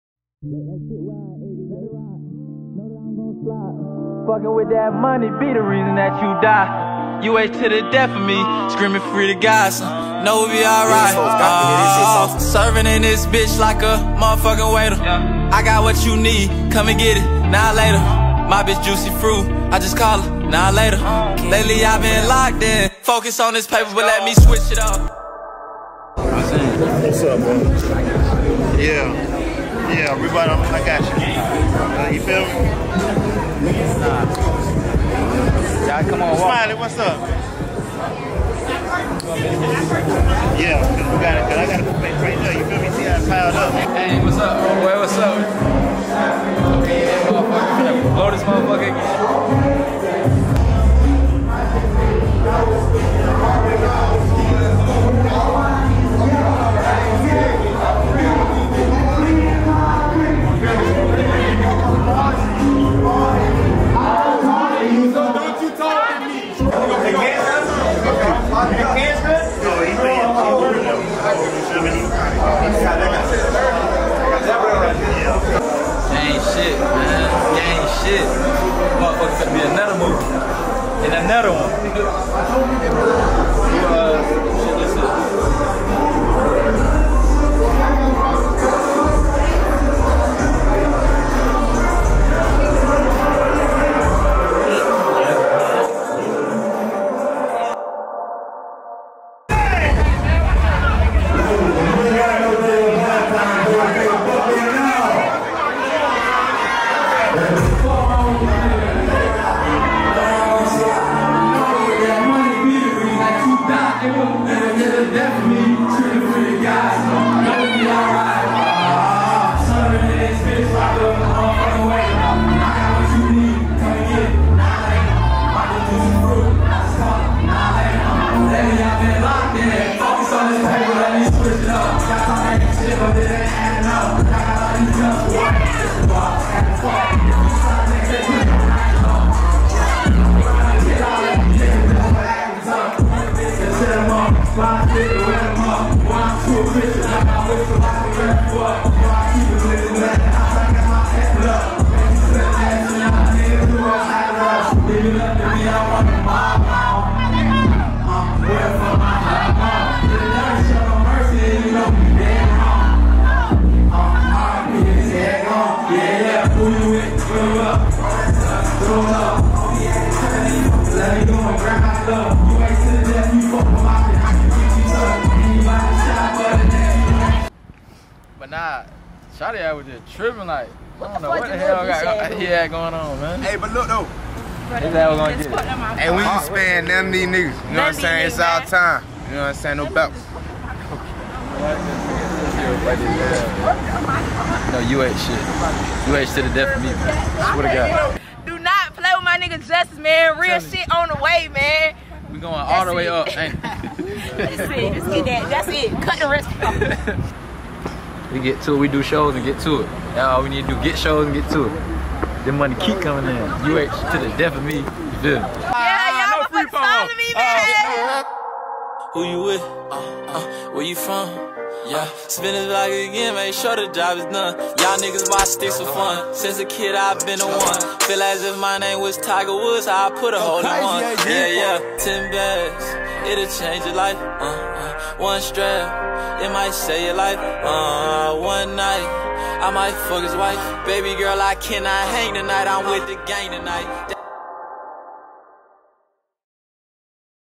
UH Justo - Live Performance VLOG UHUSTLER
In this new VLOG, UH Justo performed live and almost got into a brawl on stage before the show!

VLOG LIVE PERFORMANCE

ALL SOCIAL MEDIA LINKS AND SONGS

SECOND CONCERT!